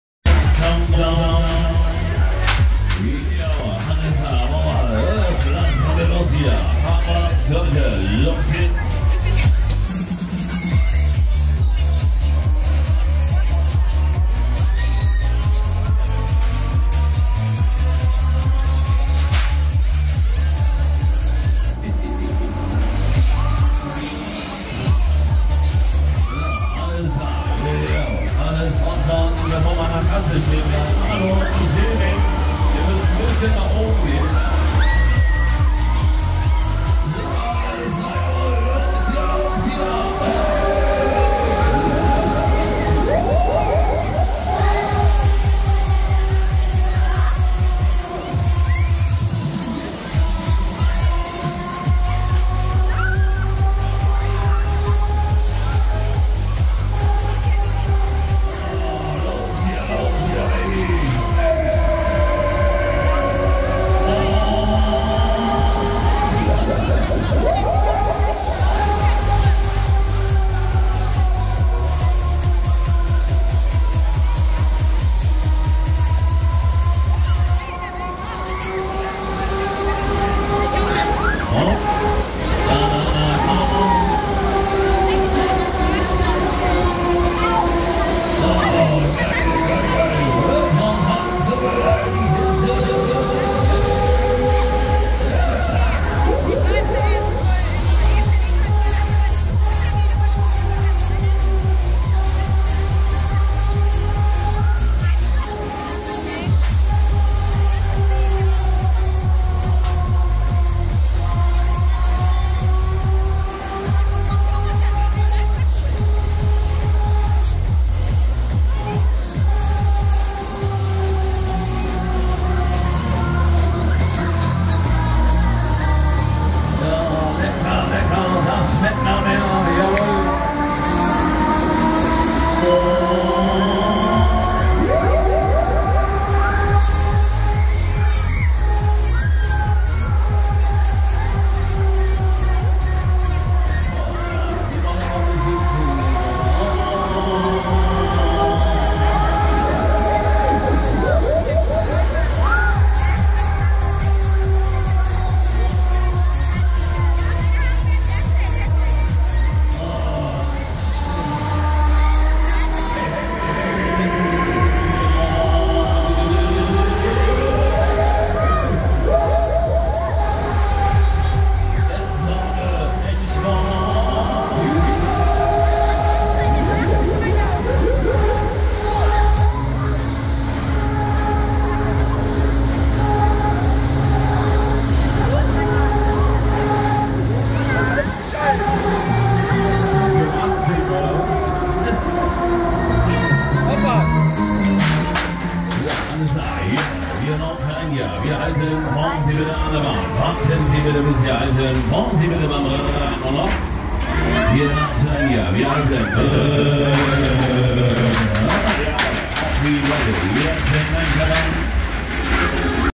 Break Dance No 1 (Dreher-Vespermann) - Stoppelmarkt Vechta 2024
Schausteller: Dreher-Vespermann GmbH & Co. KG (Bremen)

Hersteller: Huss (D)

Typ: Break Dance 1 (Nr. 1)

Baujahr: 1985

Maße (BxTxH): 19,8 x 20,1 x 11 m

Anschluss: 125 kW

Kapazität: 32 Personen in 16 Gondeln

Vorbesitzer: Dreher-Zarnitz (Bremen)

Premiere: Bremen, Osterwiese 1985

Ehem. Namen: —